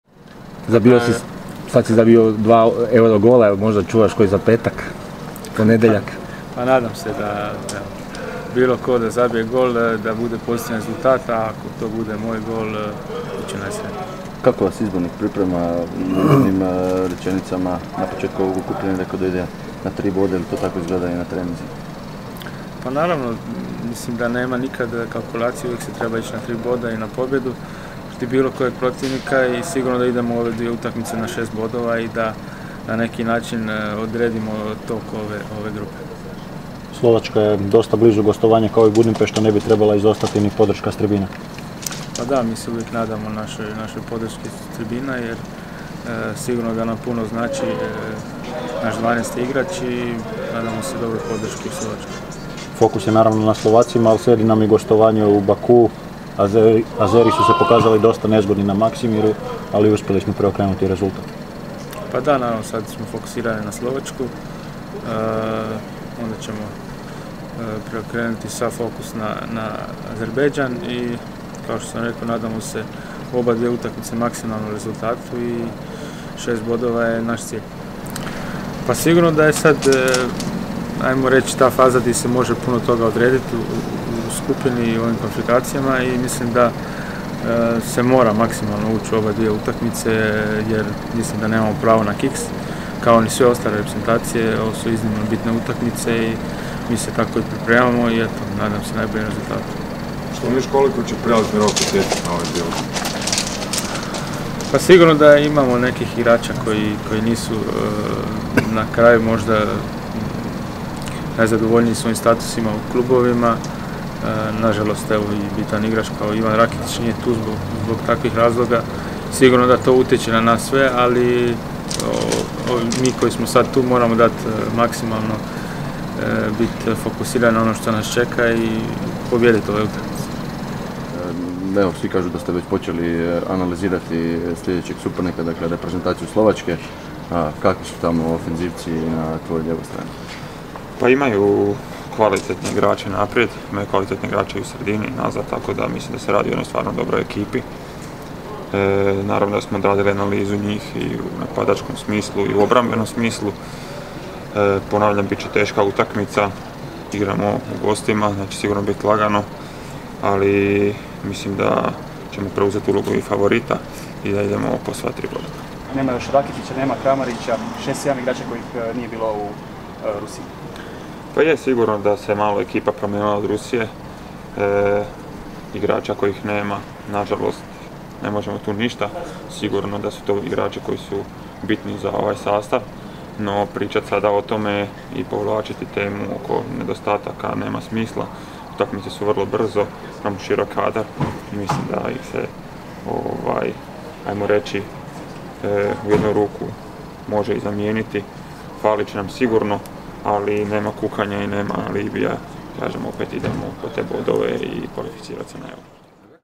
Josip Brekalo i Borna Barisic dali izjavu za medije u mix zoni
Zagreb, 040919.
Teren Hitrec Kacijan.
Trening hrvatske nogometne reprezentacije.
Snimio: Tomislav Kristo / CROPIX